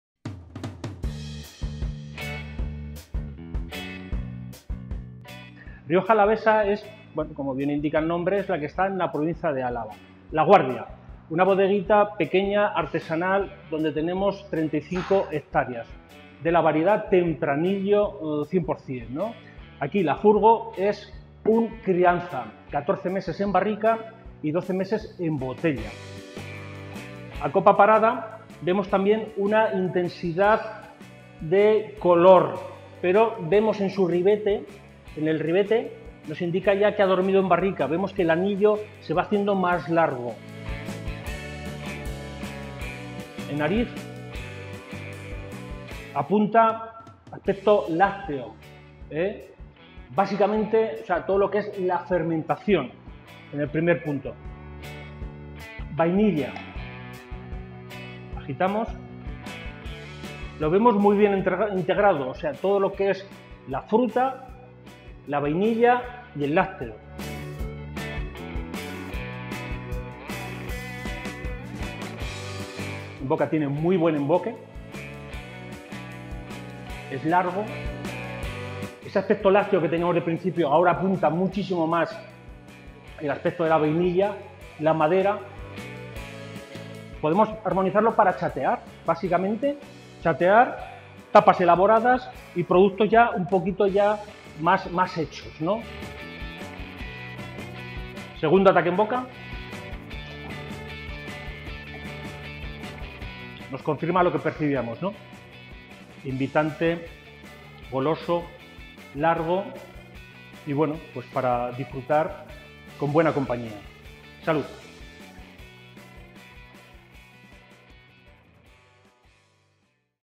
Cata Exprés /// Vino Tinto La Furgo Crianza
El Vino Tinto La Furgo Crianza pertenece a los viñedos y bodegas Vinos Divertidos y tiene una Denominación de Origen Rioja Alavesa de España.

Está compuesto por uva Tempranillo 100%. Tiene una crianza de 14 meses en barrica Francesa y 12 meses en botella.

¿Quieres probarlo? Puedes conseguirlo en nuestra página web